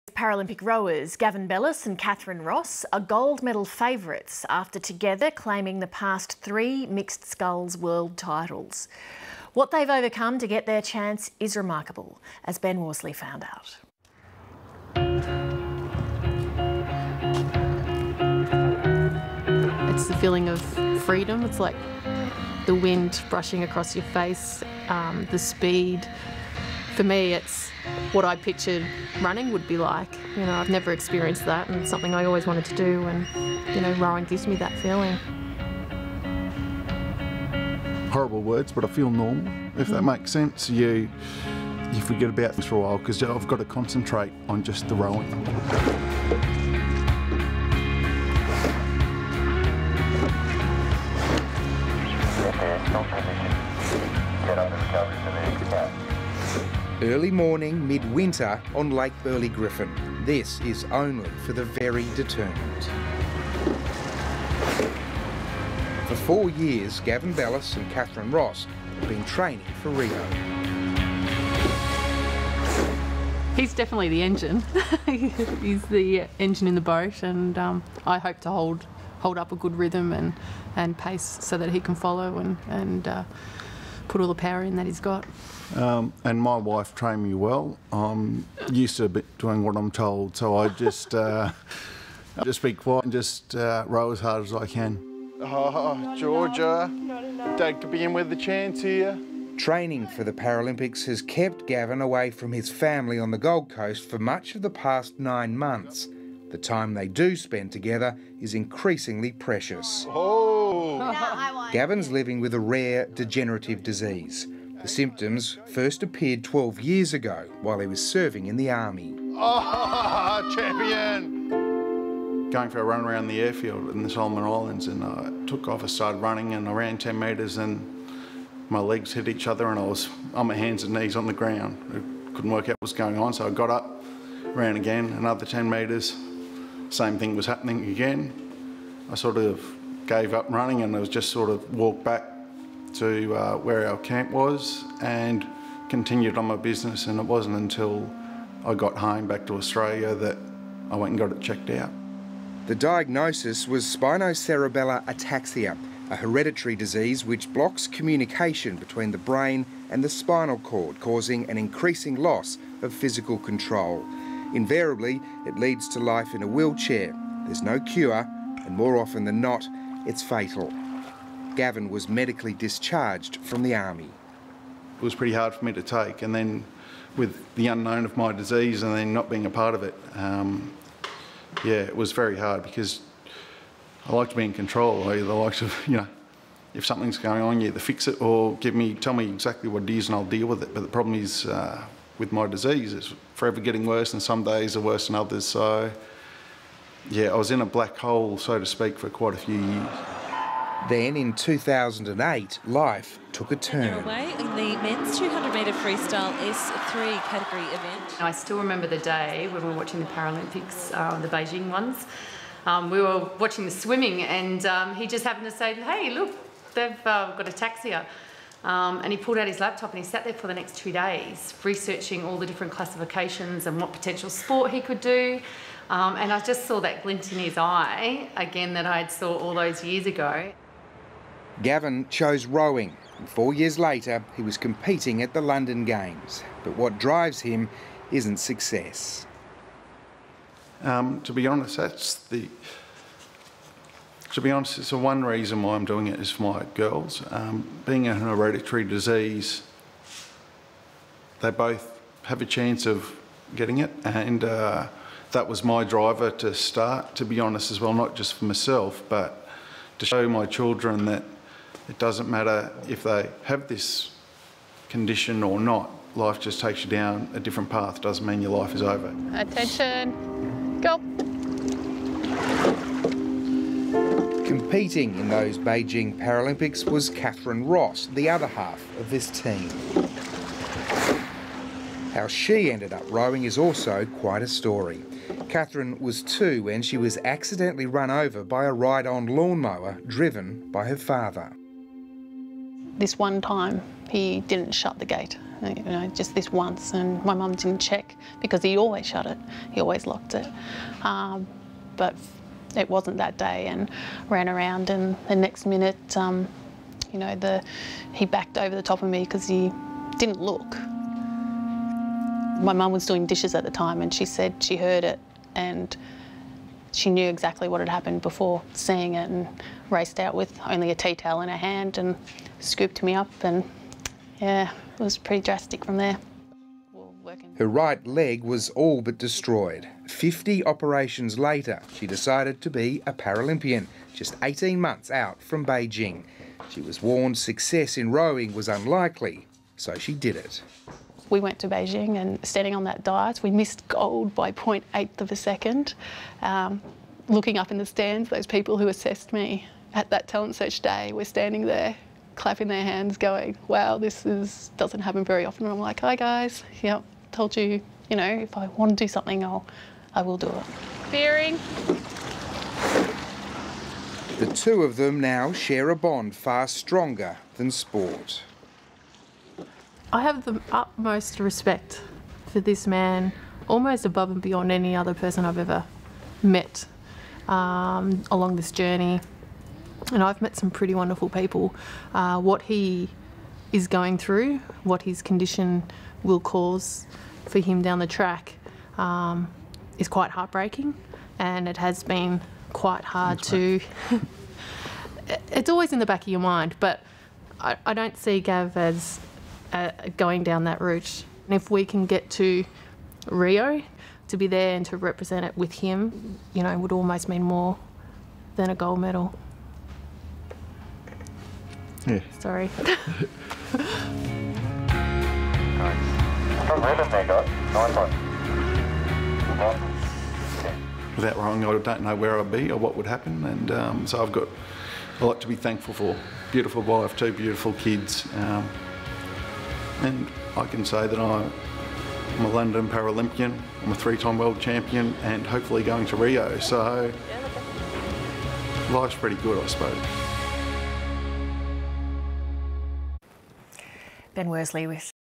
Para Olympians Kathryn Ross & Gavin Bellis
This is a story on Para Olympians Kathryn Ross & Gavin Bellis. Ross has lost a leg and Bellis has spinocereballar ataxia. This story went to air on the 10th of July 2016 ABC TV. If you need to talk to someone you can call lifeline on 13 11 14 or beyond blue on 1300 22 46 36.